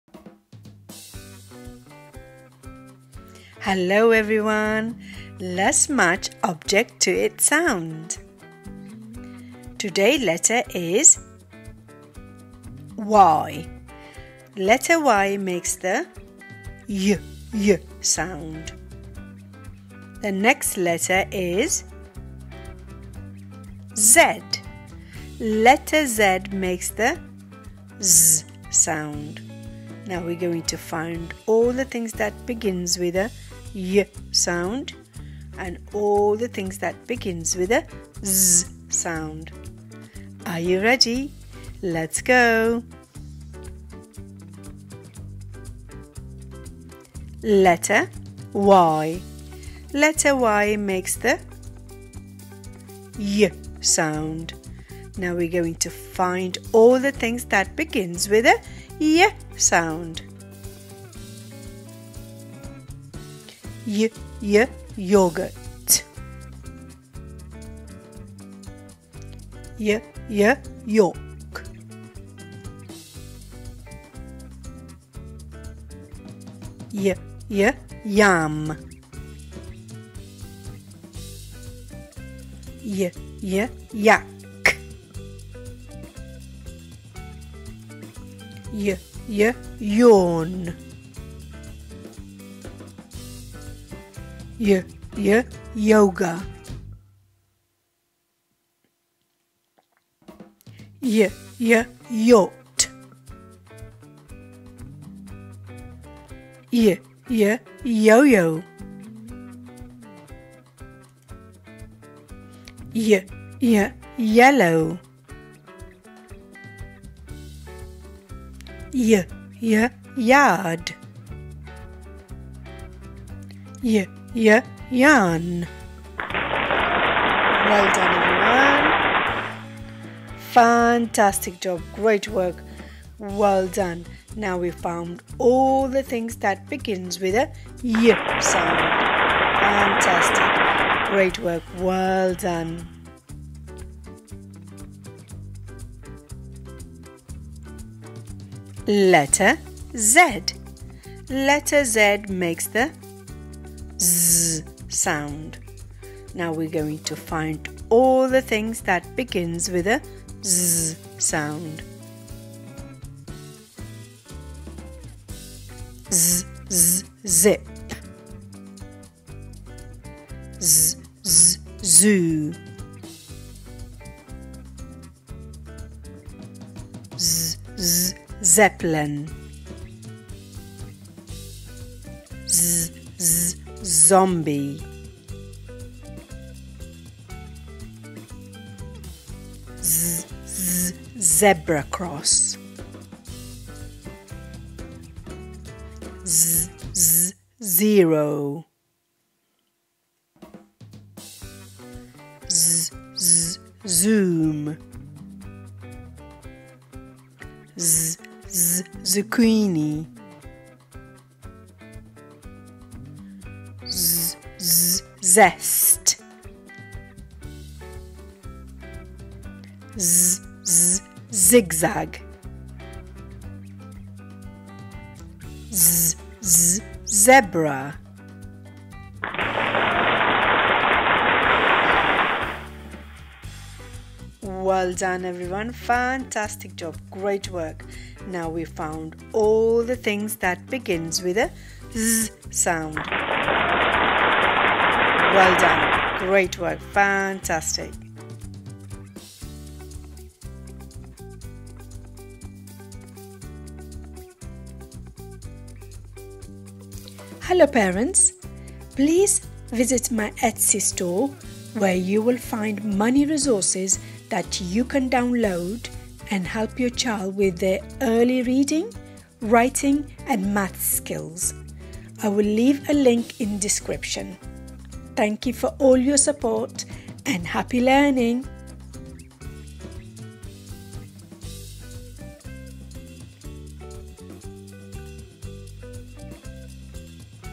Learn Letters Ww and Xx: Fun with Initial Sounds and Objects!
Learning the alphabet and recognizing the initial sounds of words is a crucial first step in literacy development. Our mats make this foundational learning experience both fun and effective, helping children to effortlessly associate letters with sounds and objects